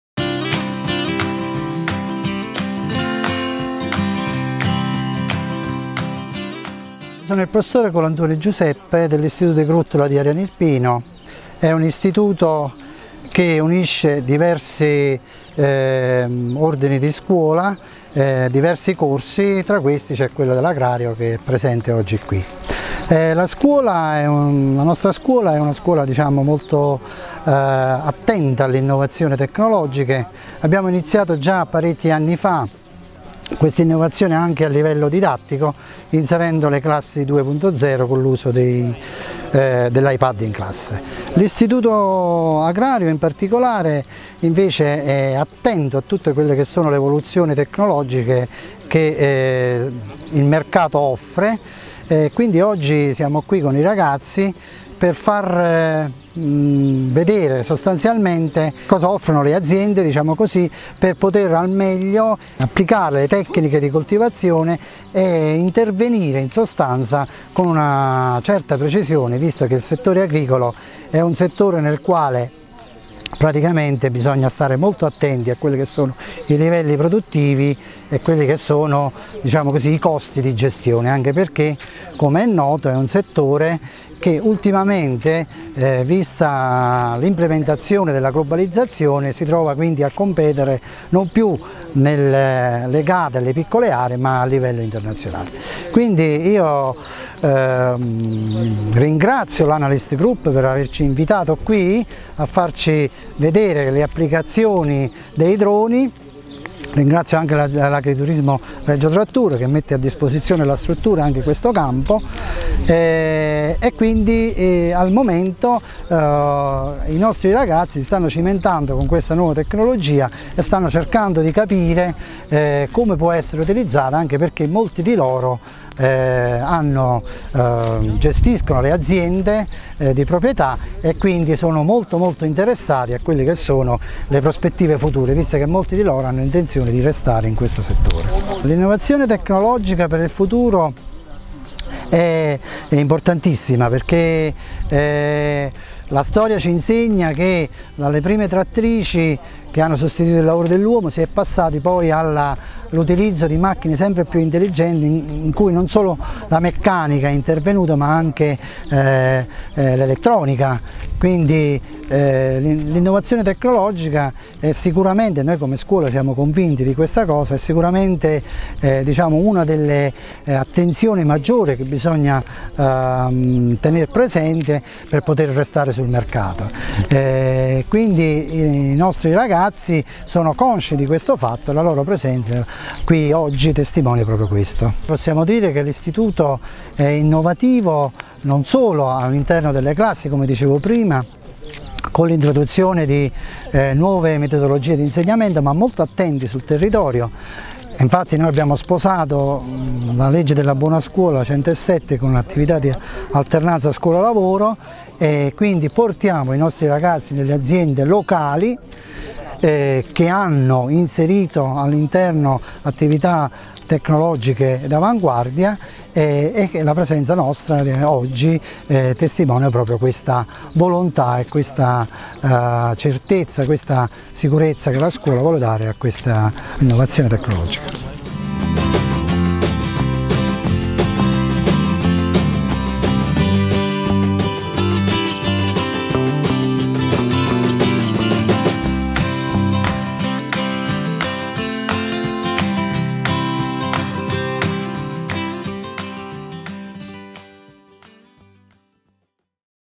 Droni Agricoltura: l'IISS "Giuseppe De Gruttola" di Ariano Irpino è innovativo
- Droni speciale Agricoltura: intervista al Prof. Giuseppe Colantuoni dell'Istituto d'Istruzione Superiore "Giuseppe De Gruttola" di Ariano Irpino (Av).

Link Utili